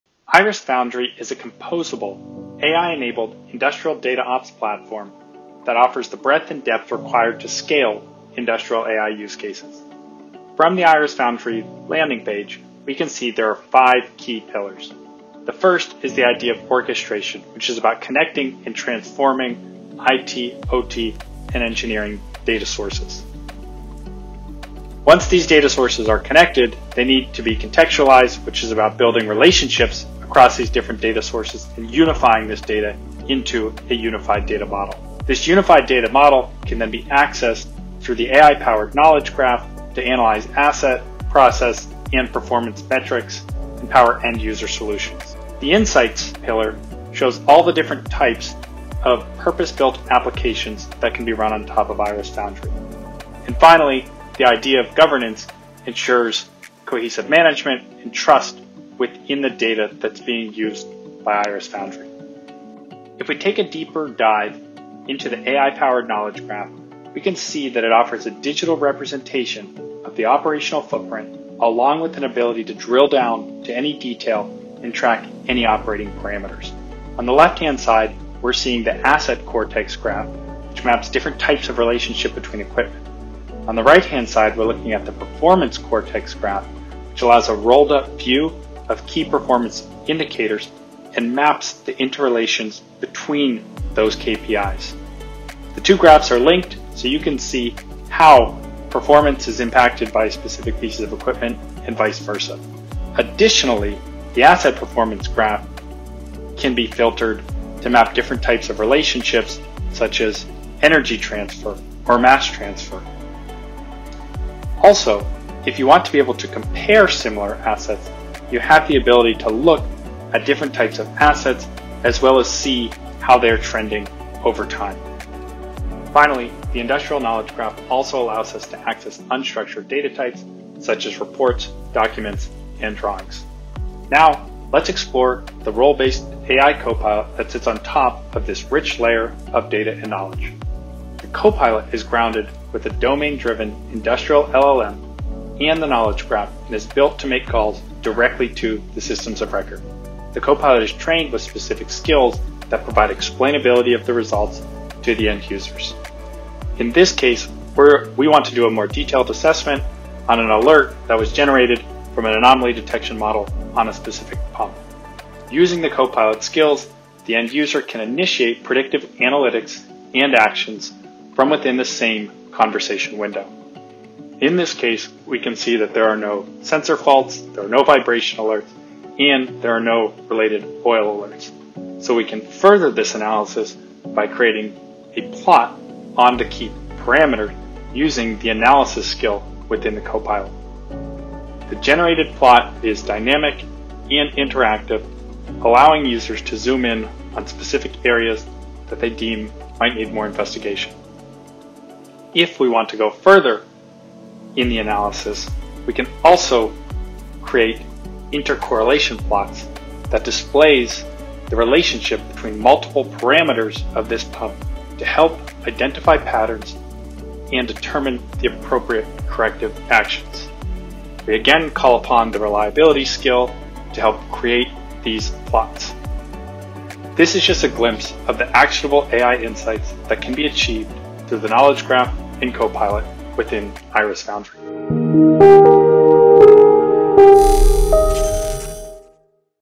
IRIS Foundry knowledge graph and Copilot for AI-powered industrial insight
Scale industrial use cases such as predictive analytics, batch optimization, energy optimization, connected worker, and predictive maintenance with industrial data operations platform, IRIS Foundry. IRIS Foundry utilizes an AI-powered knowledge graph to transform and contextualize industrial data, providing a comprehensive digital representation of operations. The IRIS Copilot is powered by a domain-driven industrial LLM, enabling predictive analytics and interactive problem-solving directly within the system. Together, these capabilities empower end users with explainable insights and actionable intelligence to optimize industrial operations.